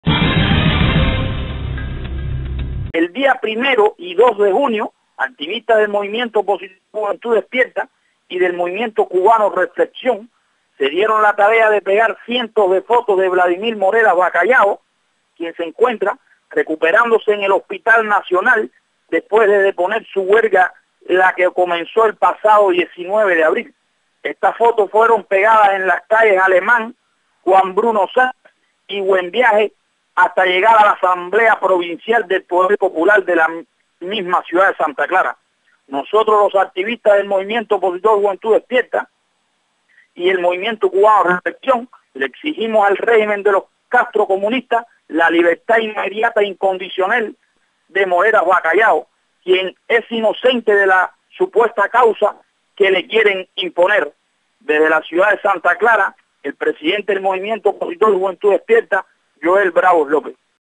Colocan pegatinas en lugares públicos exigen libertad de Vladimir Morera Bacallao
Santa Clara, Villa Clara- 4 de junio del 2015- Activistas de DDHH del Mov imiento Juventud Despierta y Movimiento Cubano Reflexión realizaron una actividad de protesta pública colocando pegatinas en calles con la foto del preso político Vladimir Morera Bacallao exigiendo su libertad. Informa Yoel bravo, Presidente del Movimiento Juventud Despierta.